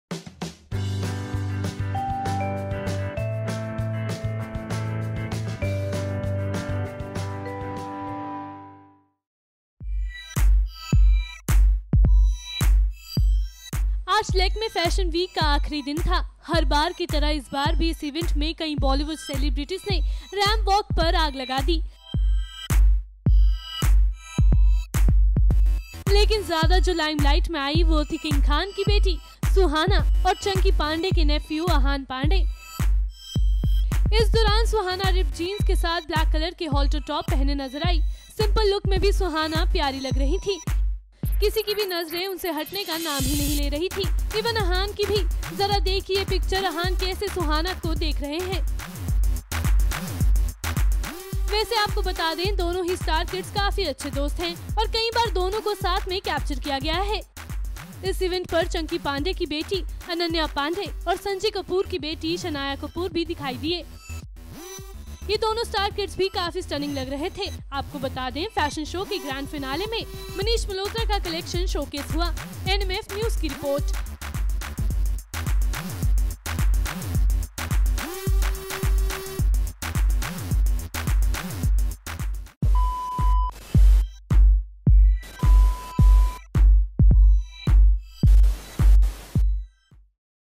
Suhana और Ahaan ने लगाए Lakme Fashion Week पर चार चांद , देखते रह गए सब
Shahrukh Khan's daughter Suhana Khan looked extremely beautiful at Lakme Fashion Week. Not just that! Suhana Khan was spotted with Ahaan Pandey at LFW. Suhana even gave poses to photographers with smiling face. To know more watch this video.